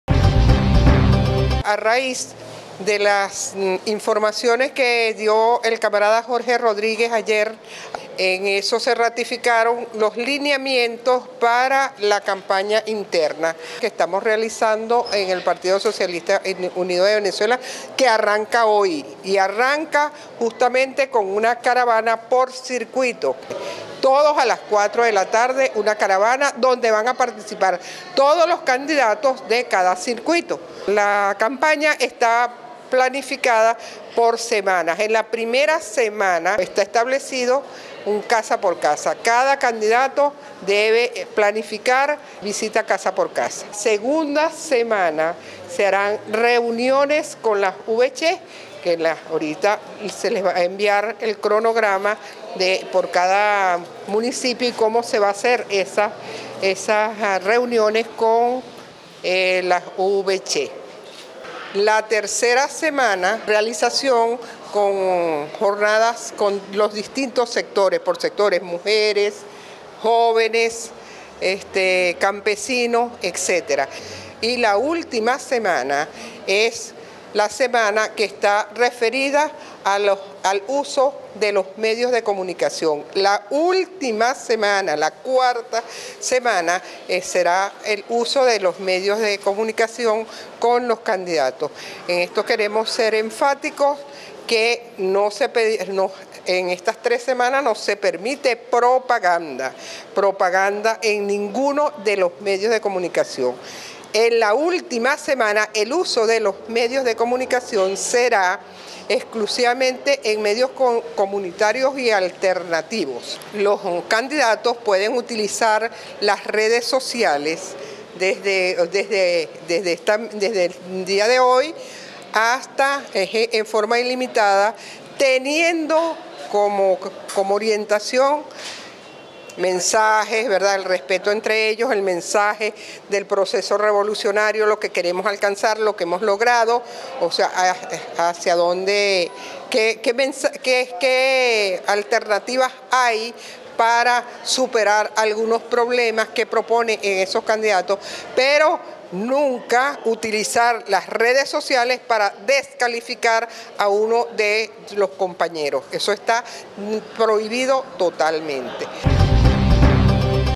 REUNIÓN PSUV PORTUGUESA.
EL PARTIDO SOCIALISTA UNIDO DE VENEZUELA DEL ESTADO PORTUGUESA SE REUNIO A FIN DE ESTABLECER EL CRONOGRAMA DE ACTIVIDADES PARA LA CAMPAÑA DE LAS ELECCIONES INTERNAS EN LA ENTIDAD LLANERA.